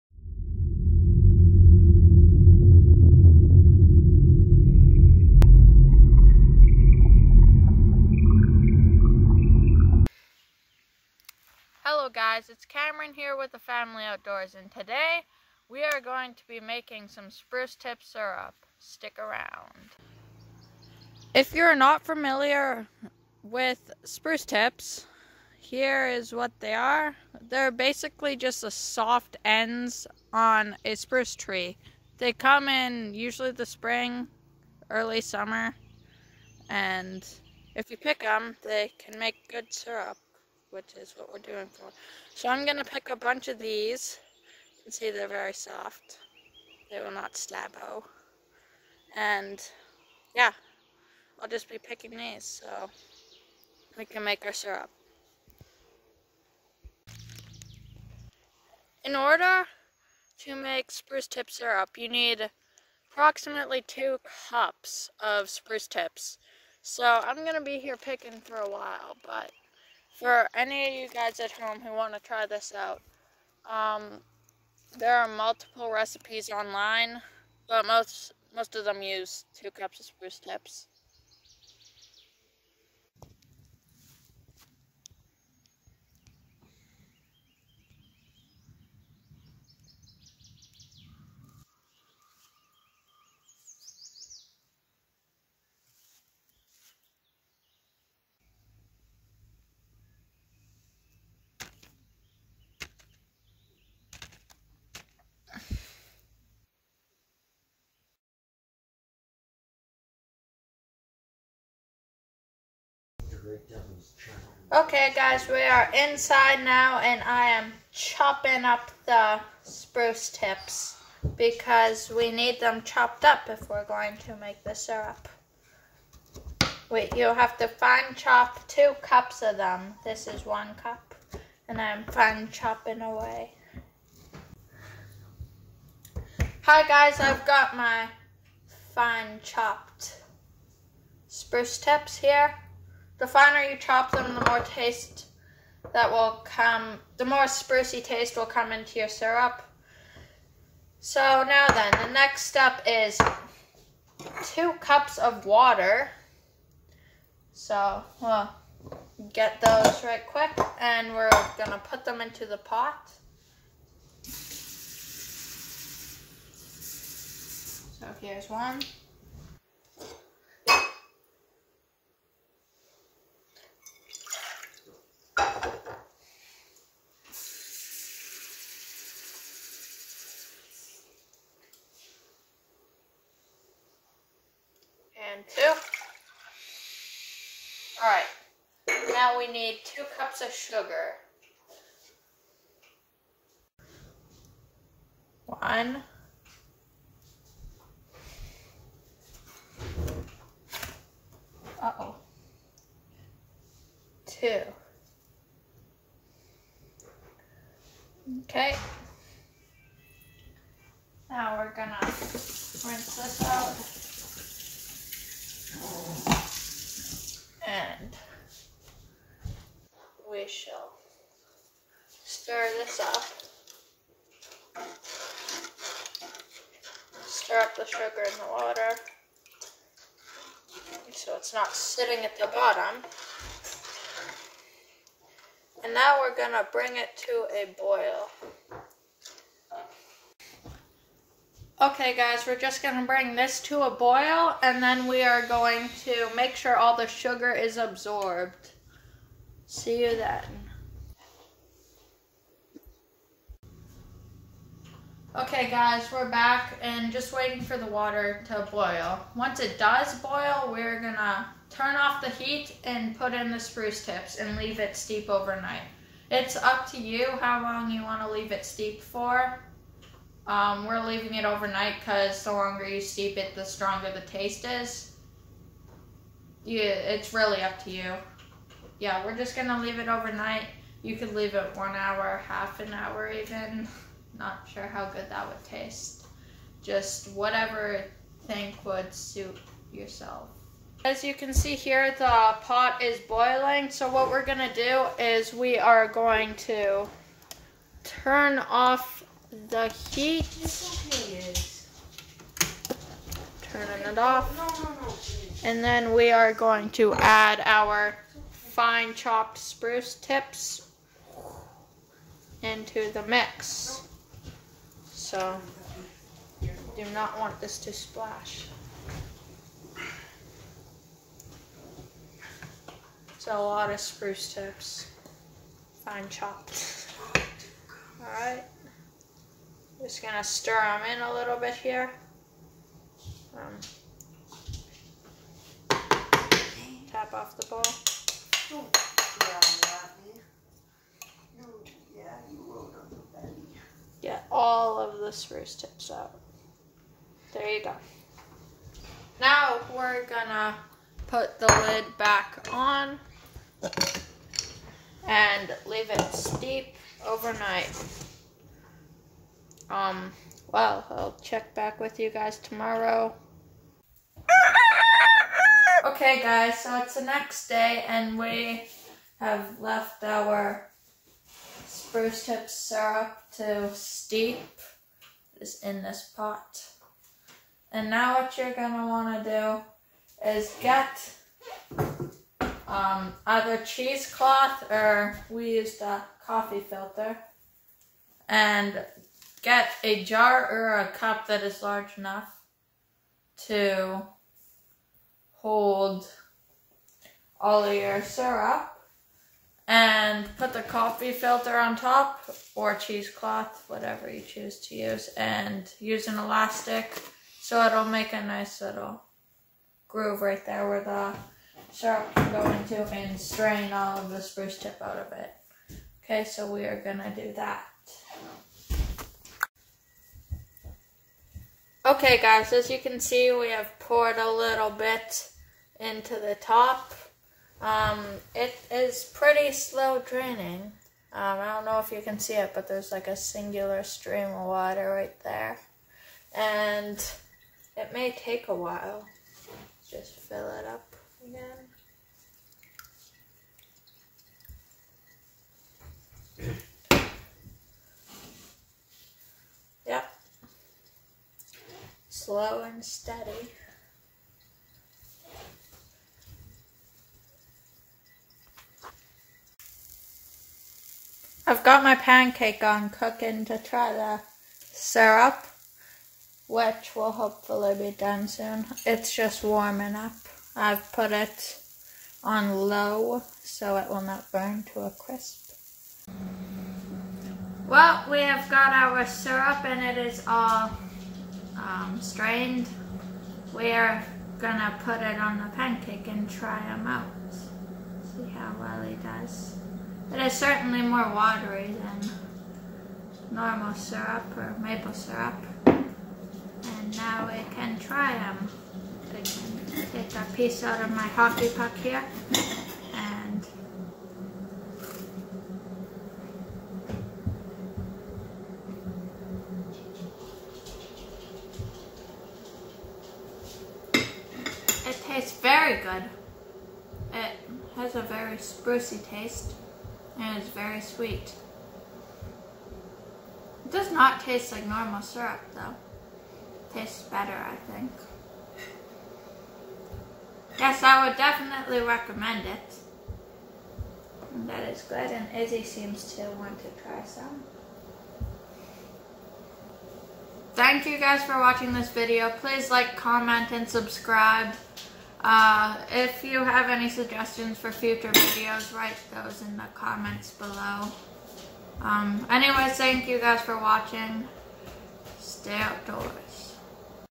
SPRUCE TIP SYRUP, WILD EDIBLES, WILDERNESS LIVING AND LIVING OFF THE LAND
In this video: Cameron shows a great way to utilize spruce tips as he walks tou through the process of making some wild syrup. Just another great way to use what's around you.